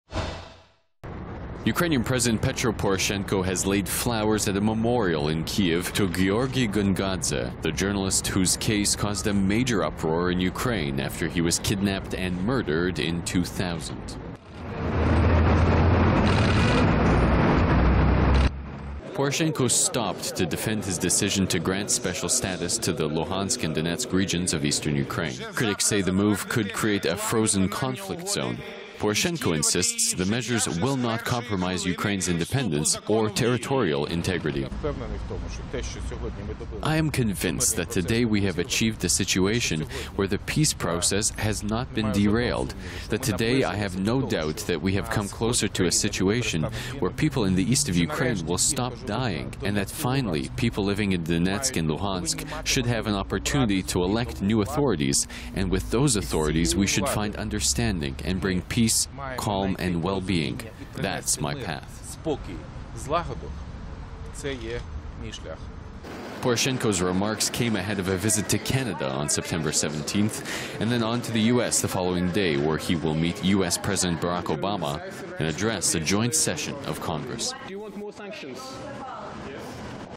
Ukrainian President Honours Killed Journalist: Poroshenko lays flowers at Georgiy Gongadze memorial
Ukrainian President Petro Poroshenko has laid flowers at a memorial in Kyiv to Georgiy Gongadze, the journalist whose case caused a major uproar in Ukraine after he was kidnapped and murdered in 2000.
Poroshenko stopped to defend his decision to grant special status to the Luhansk and Donetsk regions of east Ukraine. Critics say the move could create a frozen-conflict zone.  Poroshenko insists the measures will not compromise Ukraine's independence or territorial integrity.